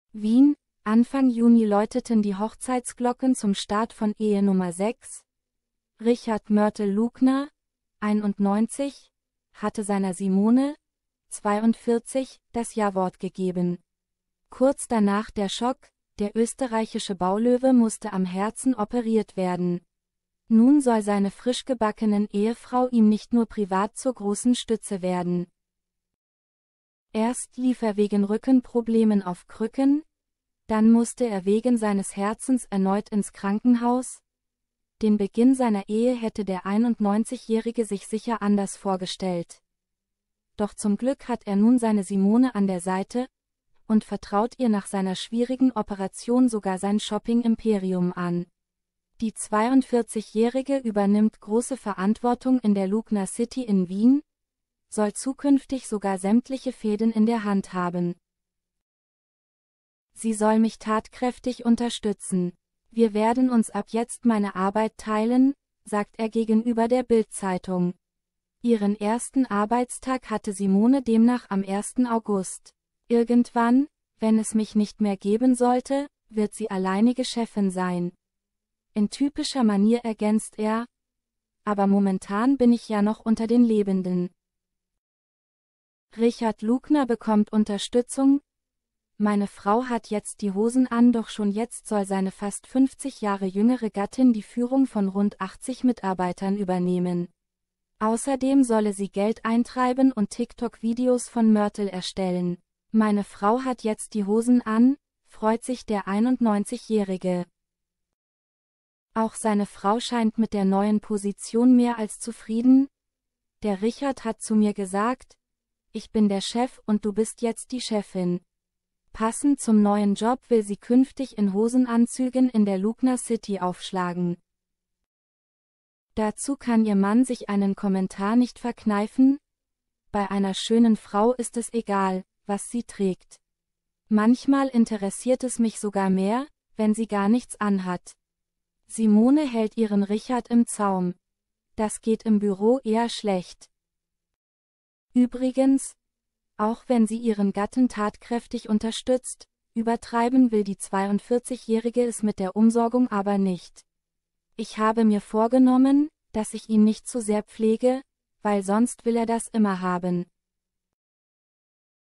Nach gefährlicher Herz OP Lugner ernennt 49 Jahre jüngere Ehefrau zur Chefin seines Imperiums
Wien - Anfang Juni läuteten die Hochzeitsglocken zum Start von Ehe Nummer Sechs: Richard "Mörtel" Lugner (91) hatte seiner Simone (42) das Jawort gegeben. Kurz danach der Schock - der österreichische Baulöwe musste am Herzen operiert werden! Nun soll seine frischgebackenen Ehefrau ihm nicht nur privat zur großen Stütze werden.